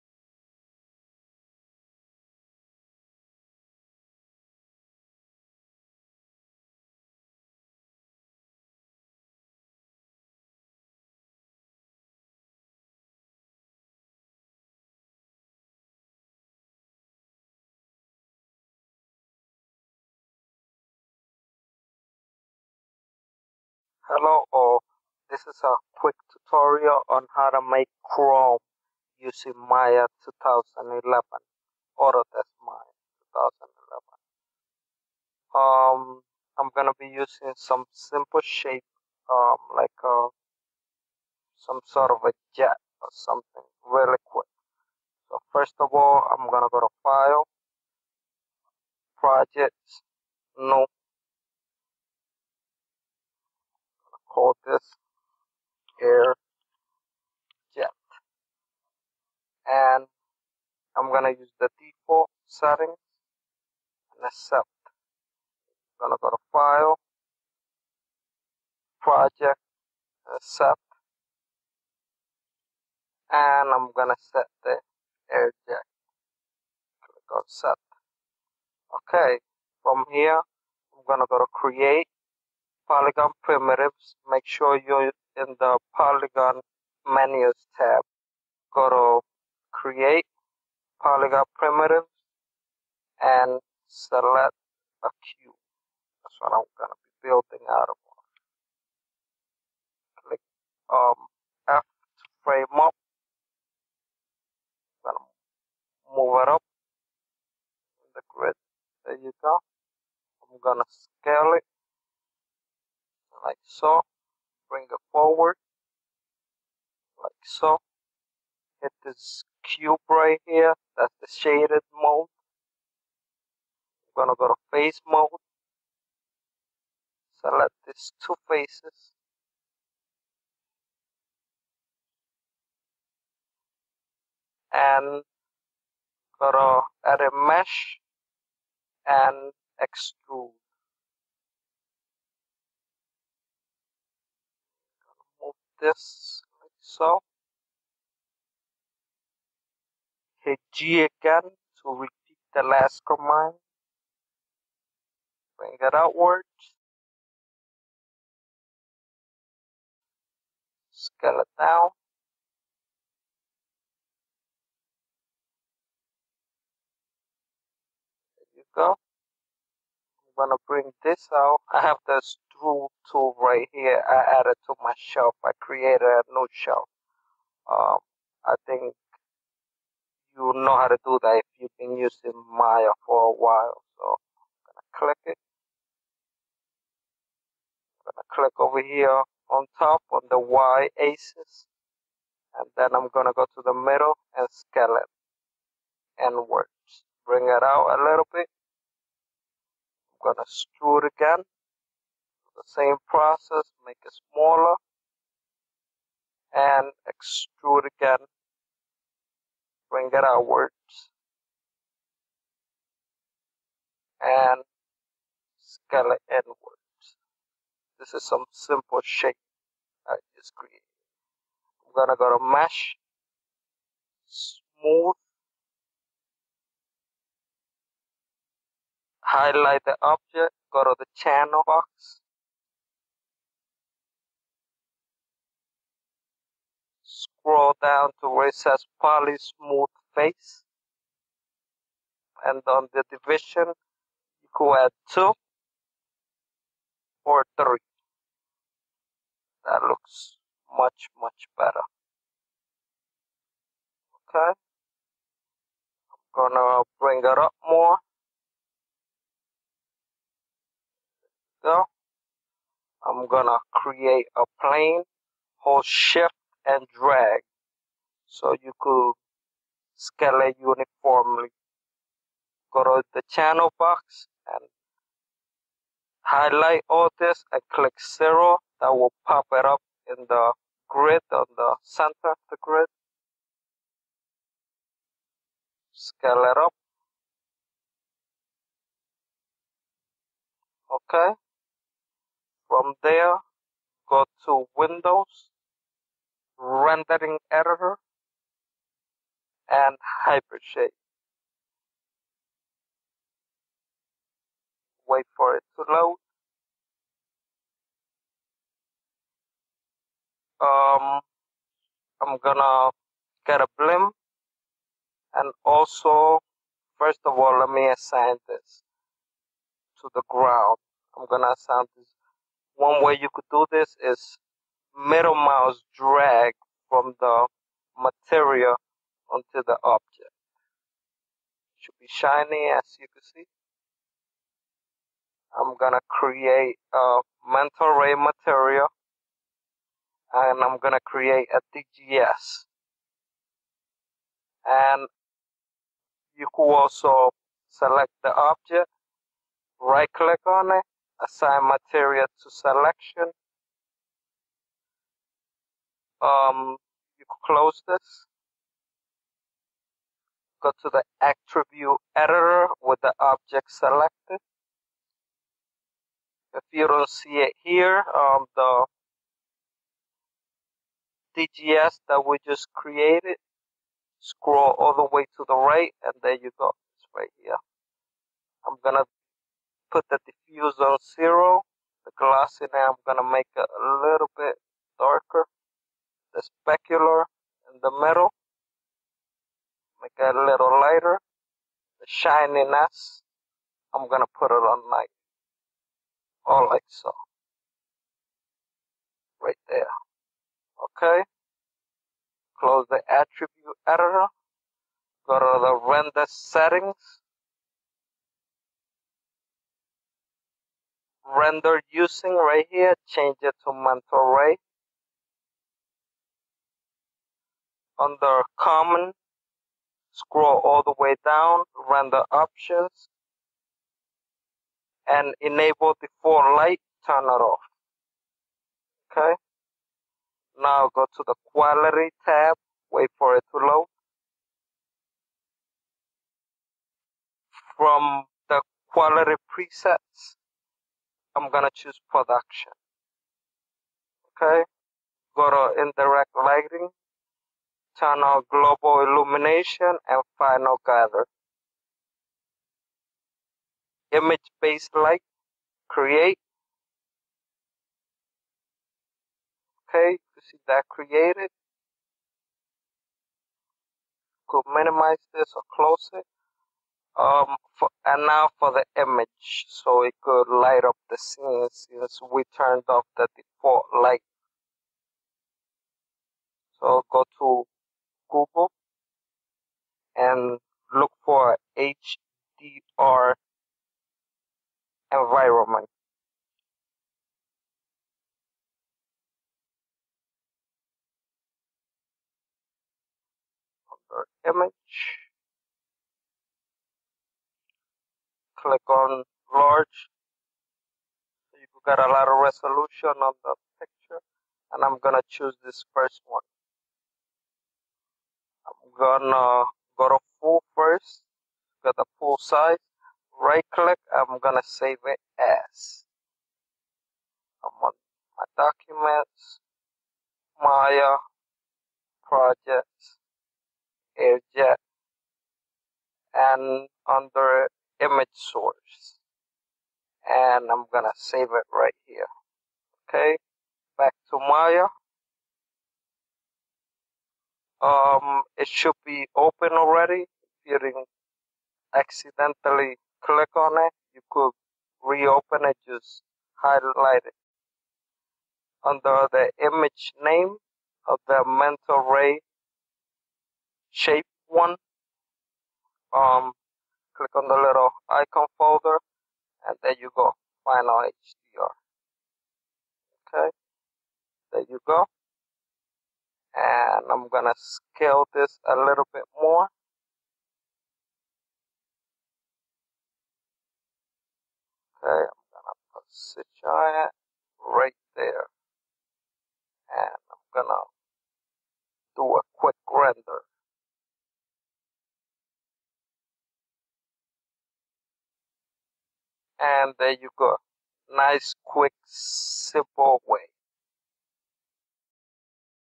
Maya 2011 Tutorial (Rendering Chrome)
This is a quick tutorial on how to make shiny Chrome in Maya 2011. Someone asked me on how to do it, so here it is.
I recommend to use Photoshop and brighten the HRD Image, so you can have a brighter daylight result.  Hope this help for you and those learning out there.